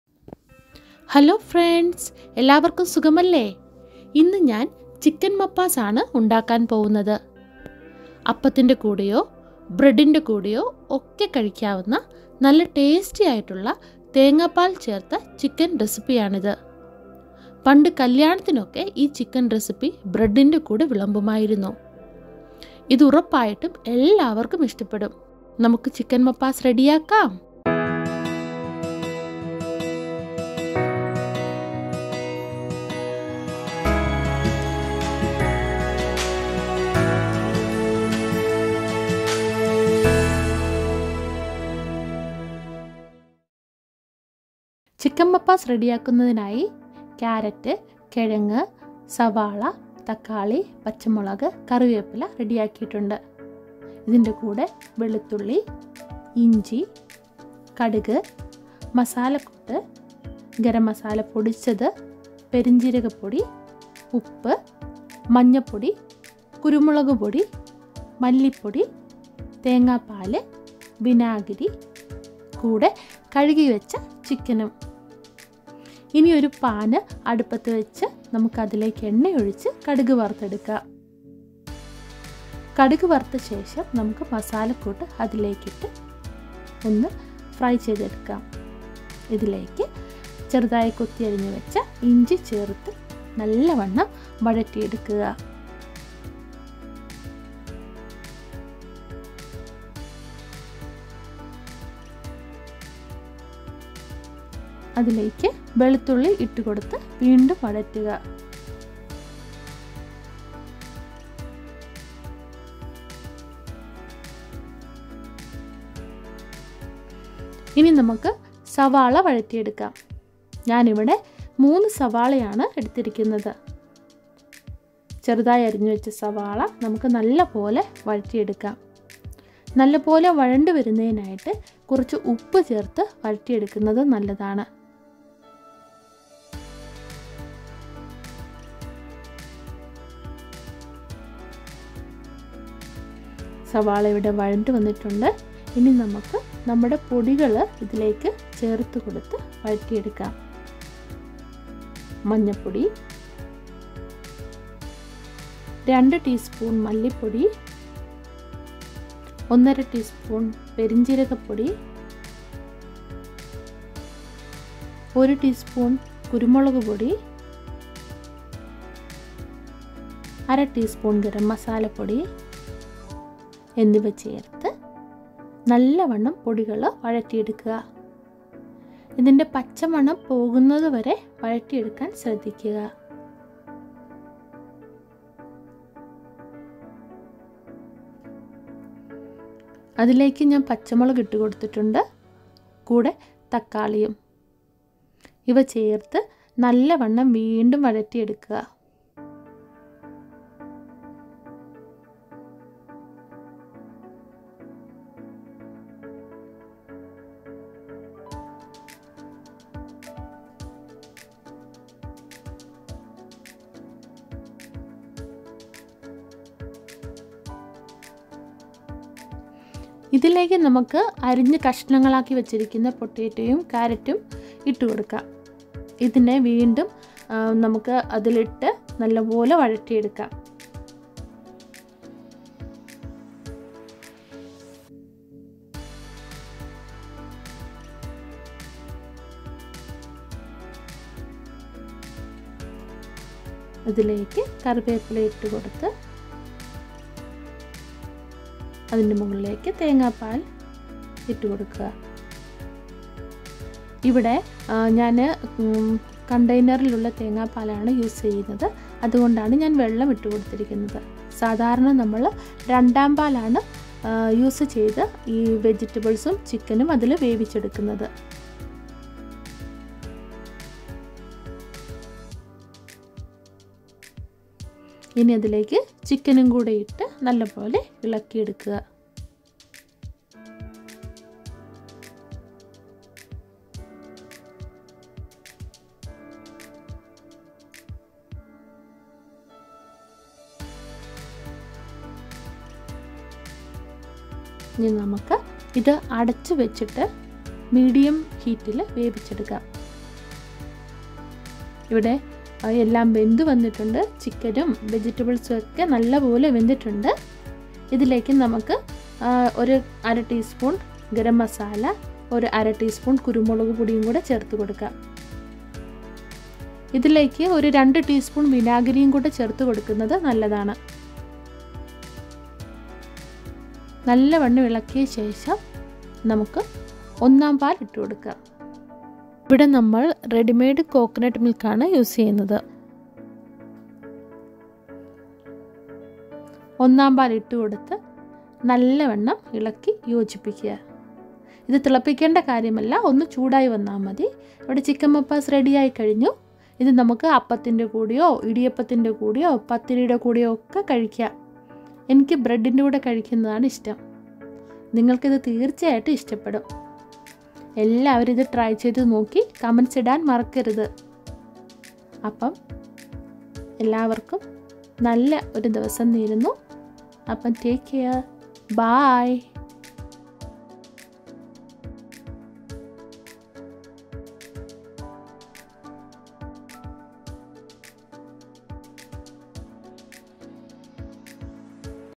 Chicken Mappas | Tasty Chicken Curry in Coconut Milk | Kerala Recipe
Ingredients:
Chicken with bone - 1.5Kg
Carrot - 2
Potato - 3
Onion - 3
Tomato - 2
Green chilli - 4
Curry leaves
Garlic - 8 cloves
Ginger
Mustard seeds
Whole spices
Garam masala - 1 teaspoon
Fennel powder - 1.5 teaspoon
Coconut milk
Vinegar - 2 teaspoon
Salt

Wishing you all the best. Thank You!